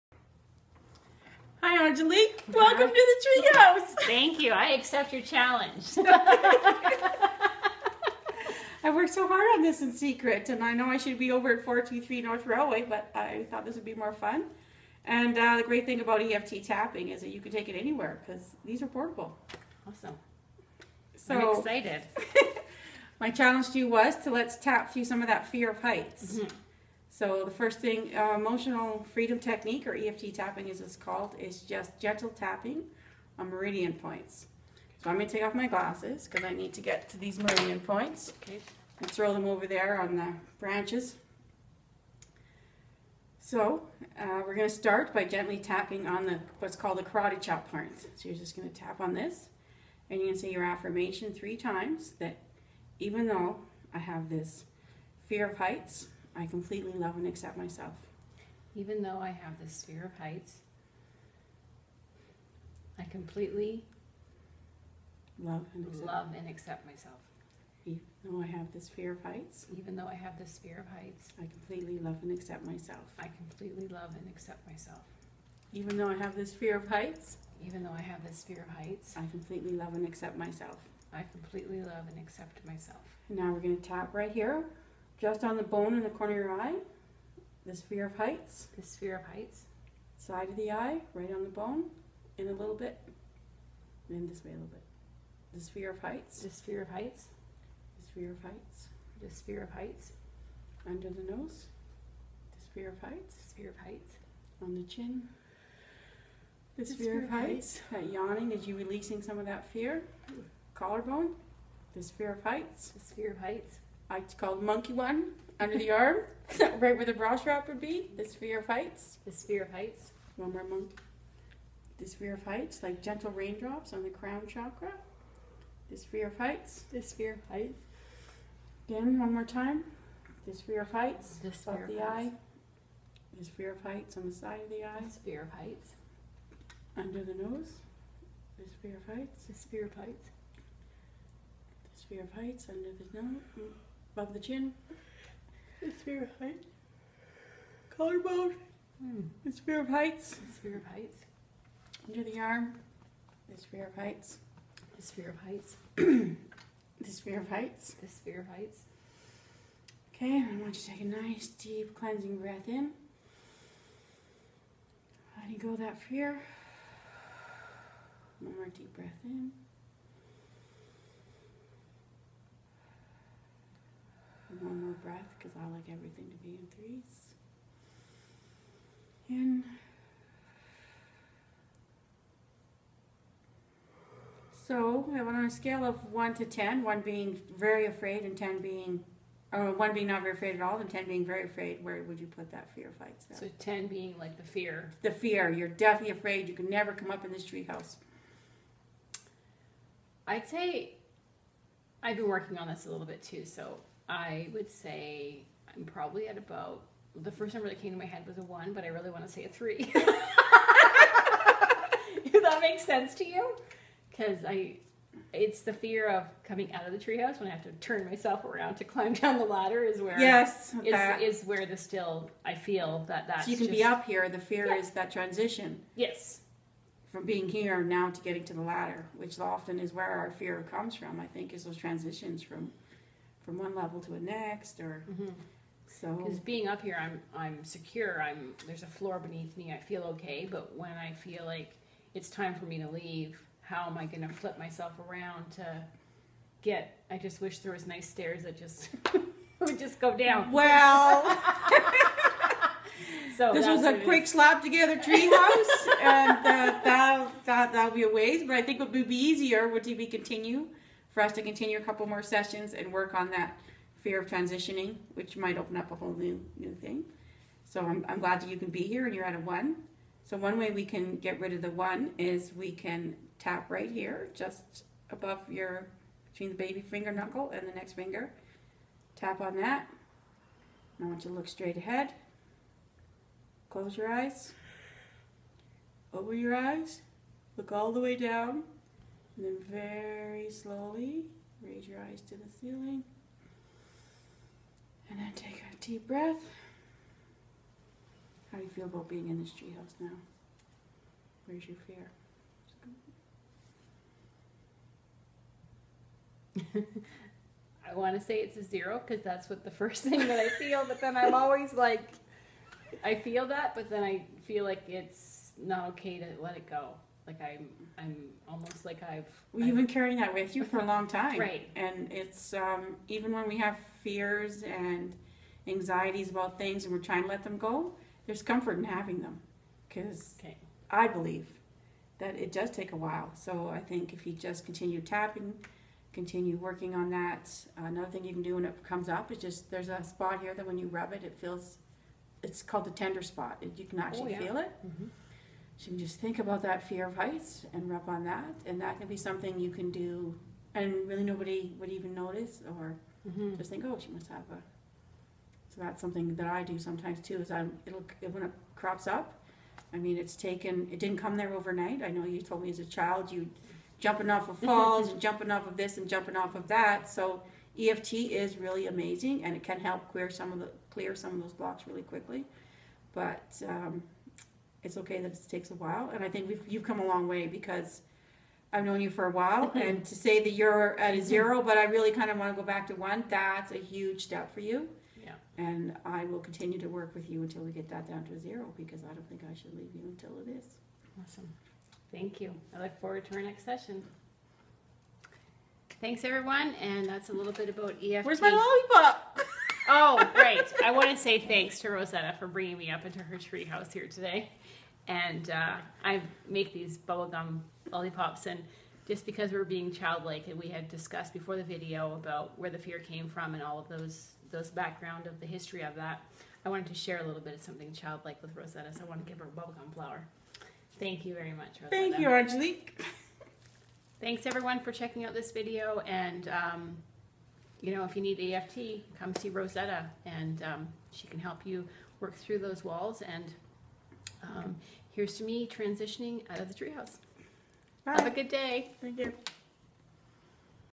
EFT-Tapping on fear
Rosetta my EFT (emotional freedom technique) coach helped me though a big hurdle. We have been working with session prior to this one and I have been doing a lot of independent work as well. Later this day I had a big break through. I stood on the kitchen counter and jumped down off the counter, with no fear, no sweaty hands, no
Crying, no heart beat racing, just acceptance that I was save and all was well. Amazing break through for me.